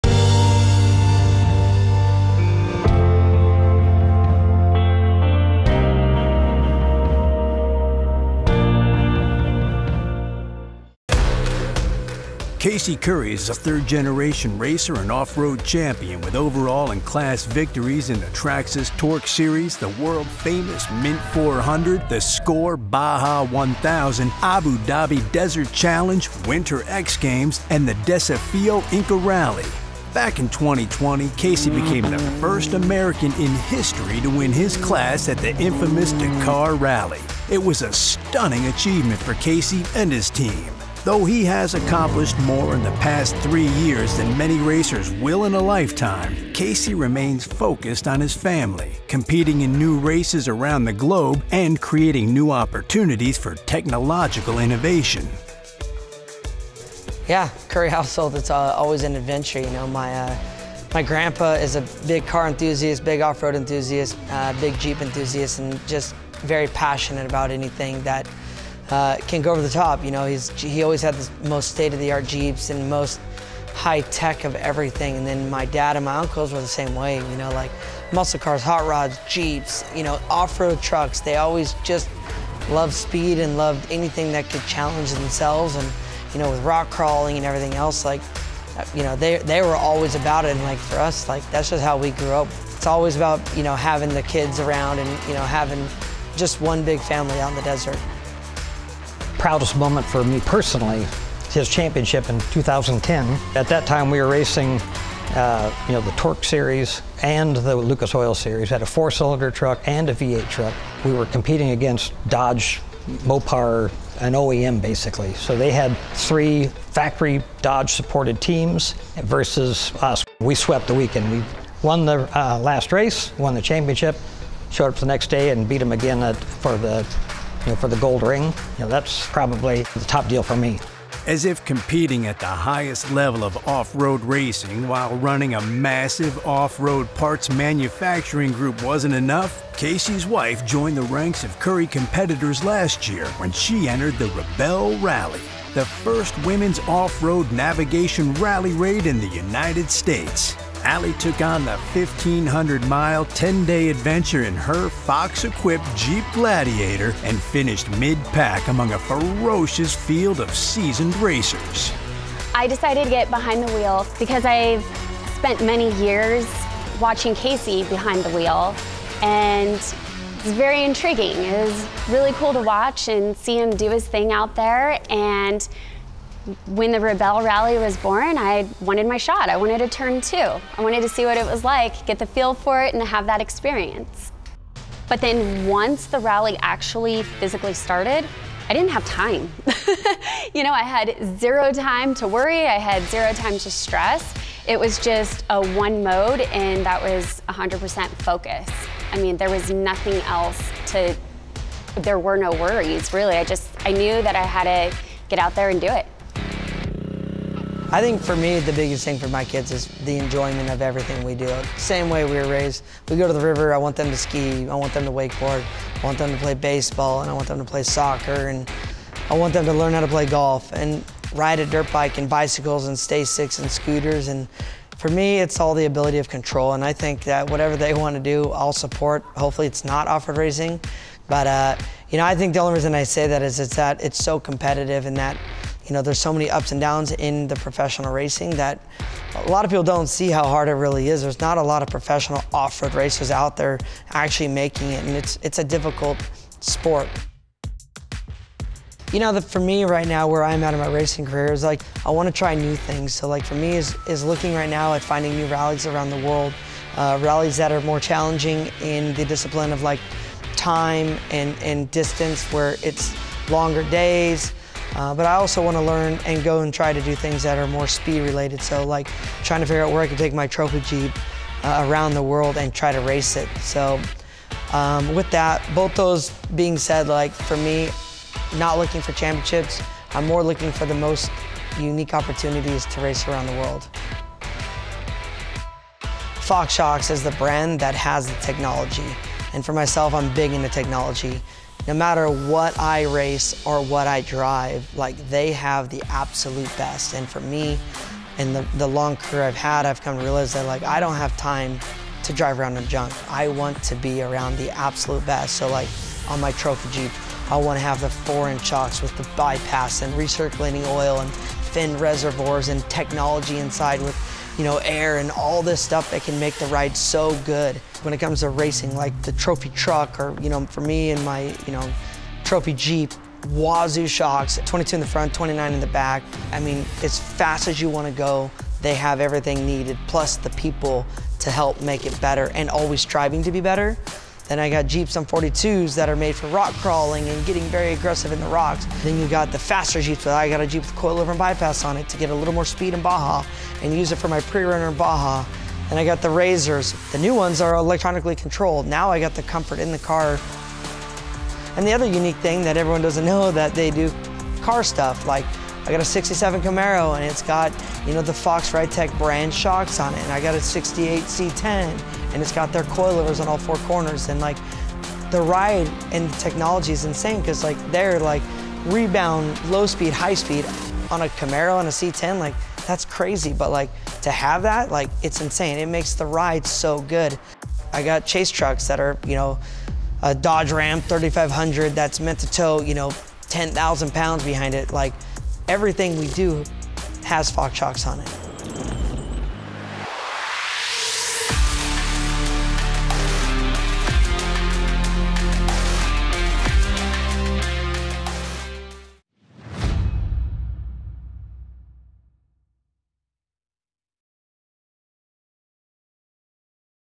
FARTHER With Casey Currie | FOX
Casey Currie is a third-generation racer and off-road champion with overall and class victories in the Traxxas TORC Series, the world-famous Mint 400, the SCORE Baja 1000, Abu Dhabi Desert Challenge, Winter X Games, and the Desafio Inca Rally. Back in 2020, Casey became the first American in history to win his class at the infamous Dakar Rally. It was a stunning achievement for Casey and his team. Though he has accomplished more in the past three years than many racers will in a lifetime, Casey remains focused on his family, competing in new races around the globe, and creating new opportunities for technological innovation.

As if competing at the highest level of off-road racing while running a massive off-road parts manufacturing group wasn’t enough - Casey’s wife joined the ranks of Currie competitors last year when she entered the Rebelle Rally - the first women’s off-road navigation rally raid in the United States. Ali took on the 1,500-mile 10-day adventure in her Fox-equipped Jeep Gladiator and finished mid-pack among a ferocious field of seasoned racers.